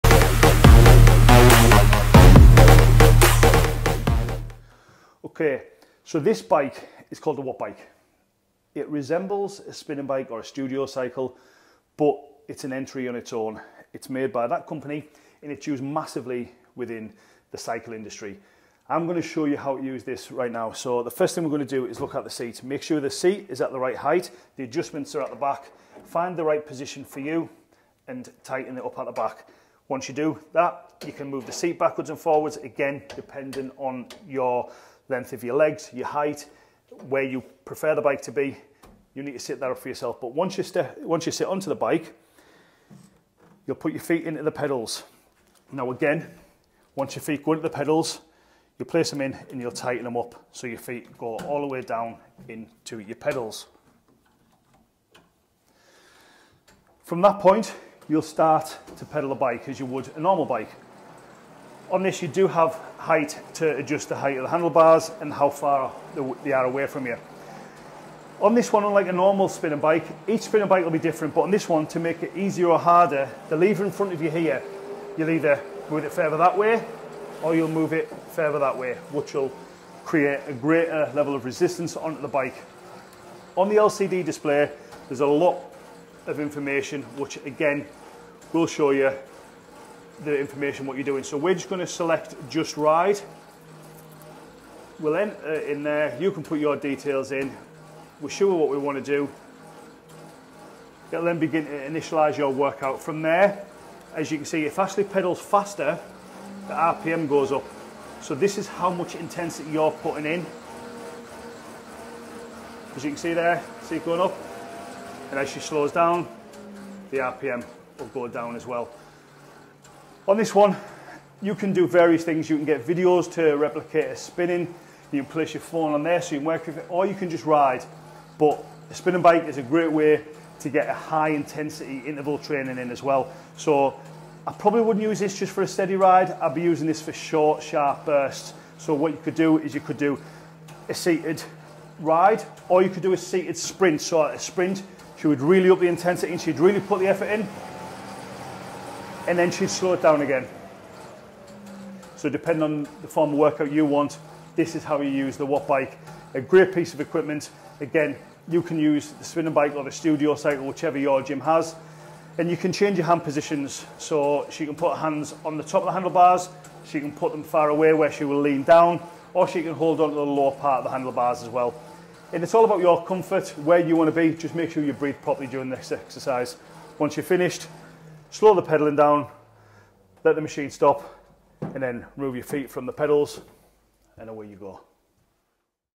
How To Use The WATT BIKE: A Step-by-Step Tutorial for Better Cardio Workouts
In this video, we'll be exploring the Watt Bike, a state-of-the-art indoor cycling machine that's taking the fitness world by storm. Whether you're a seasoned cyclist looking to improve your performance or a fitness enthusiast looking for a challenging new workout, the Watt Bike has something for you.